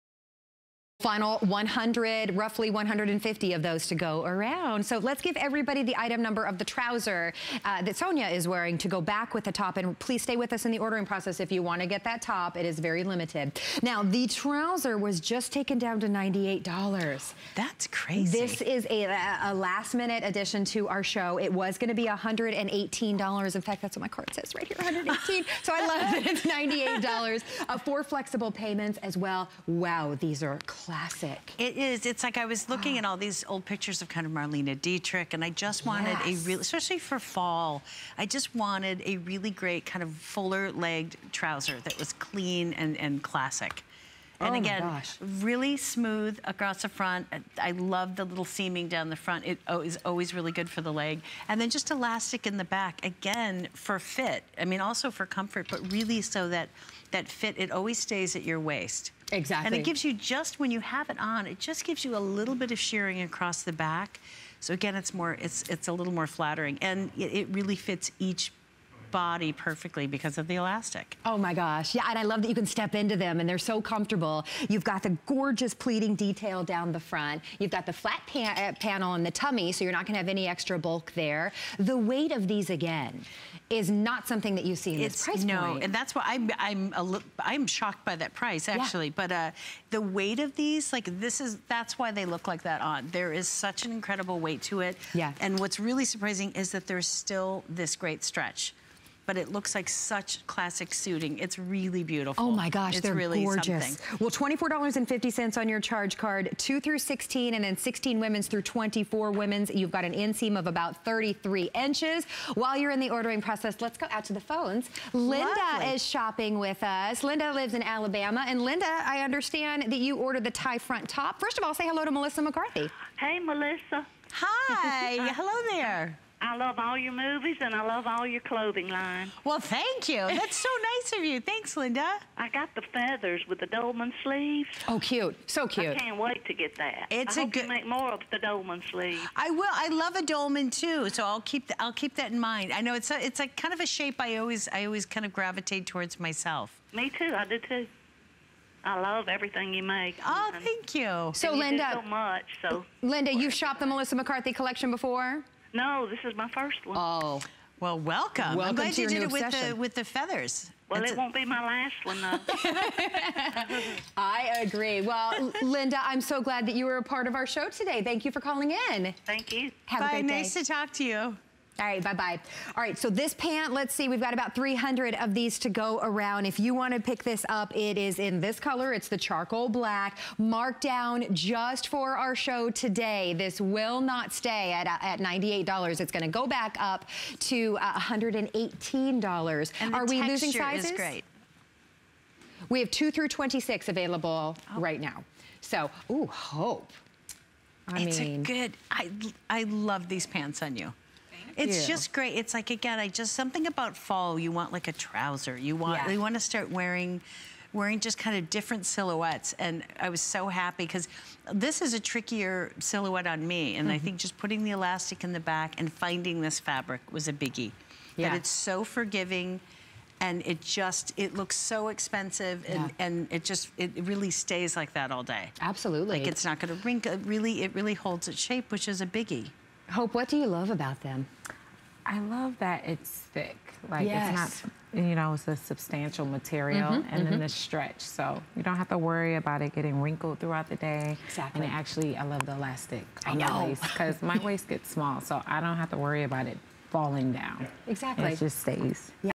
Melissa McCarthy Seven7 Permanent Crease Trouser Missy
Melissa McCarthy Seven7 Permanent Crease Trouser
A pair of tailored trousers never go out of style. This easy pullon, woven charcoal and black design features a slimming, permanent crease down...